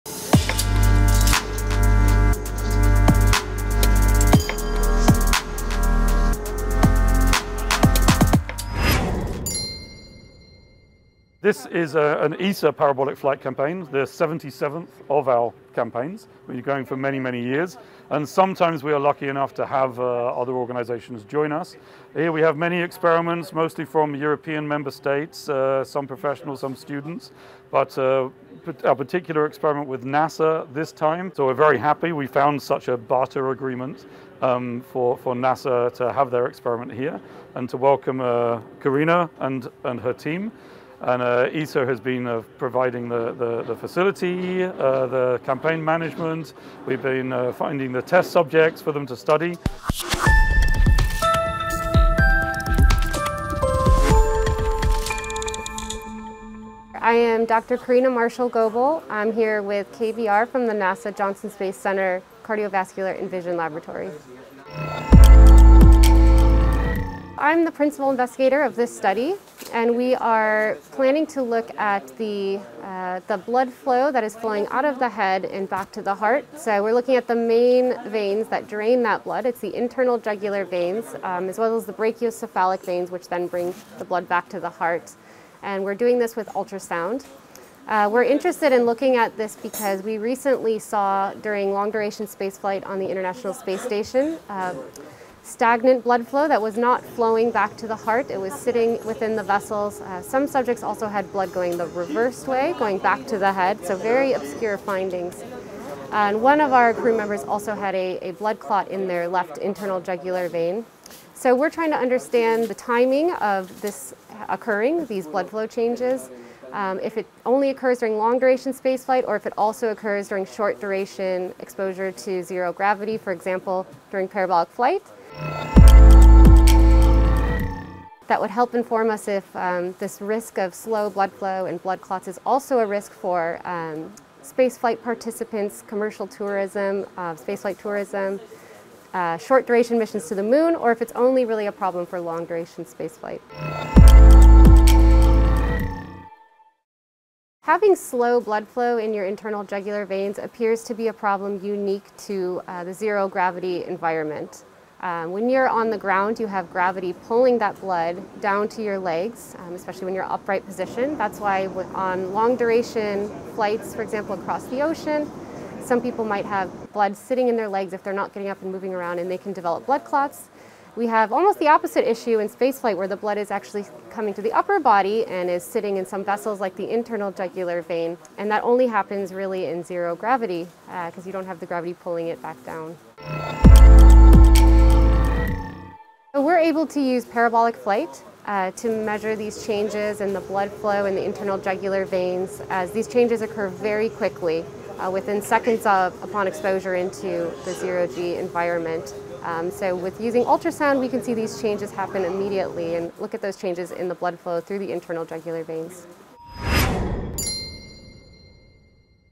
77th ESA PARABOLIC FLIGHT CAMPAIGN - Karina Marshall-Goebel - Jugular Blood Flow in Microgravity
🩸Did you know that astronauts risk thrombosis on board the ISS? Recently, Karina Marshall-Goebel's team evaluated blood flow in the jugular vein of astronauts during long-duration spaceflight missions onboard the International Space Station. Chronic exposure to weightlessness resulted in unexpected stagnant or reverse flow in 6/11 crewmembers. Thanks to an agreement with ESA during their 77th parabolic science flight campaign, NASA’s Katarina and her team studied the time course of venous blood flow stasis formation in weightlessness to further their knowledge regarding the risks of altered blood flow and indeed possibly thrombosis, whether during long-duration spaceflights or even for shorter commercial space flights. These studies are complementary to those carried out by NASA in bedrest tests.